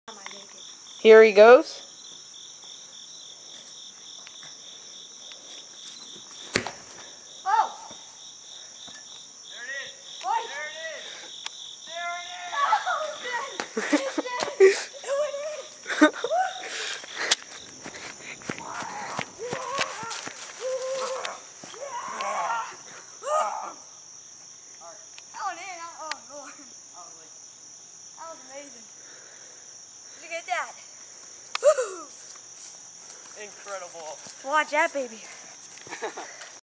Lukes Long Soccer Goal
Luke shoots a soccerball probably 100ft for Whacky Sports show #1